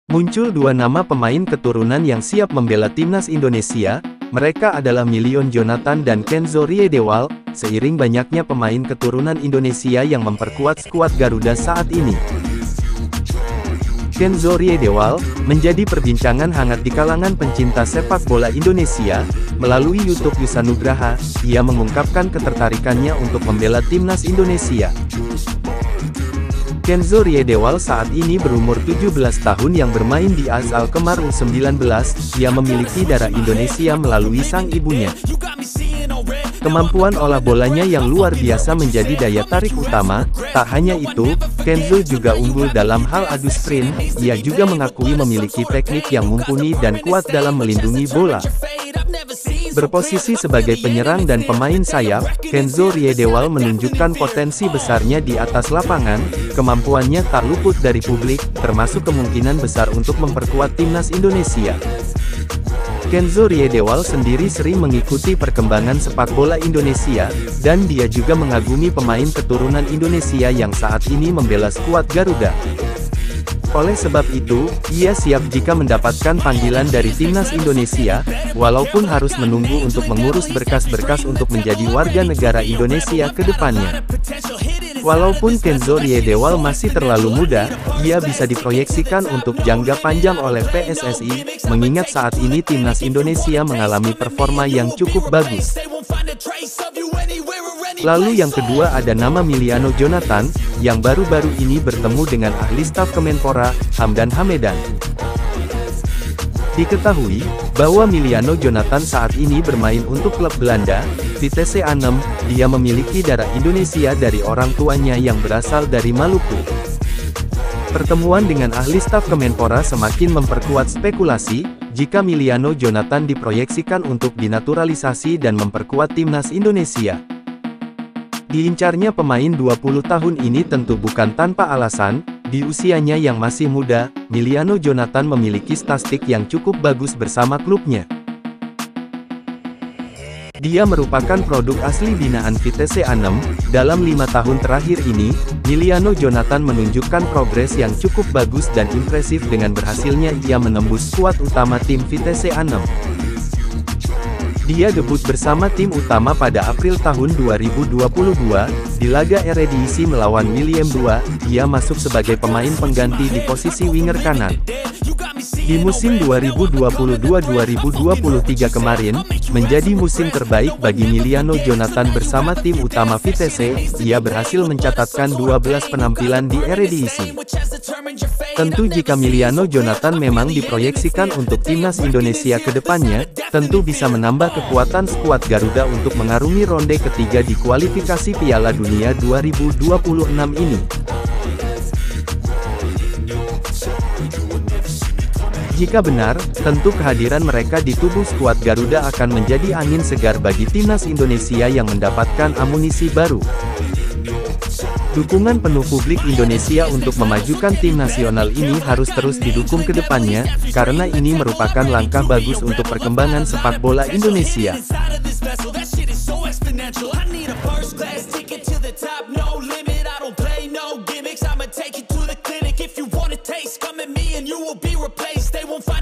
TIMNAS INDONESIA DIAM DIAM BUAT KEJUTAN MENDATANGKAN PEMAIN KETURUNAN YANG BARU!!
Pemain keturunan timnas indonesia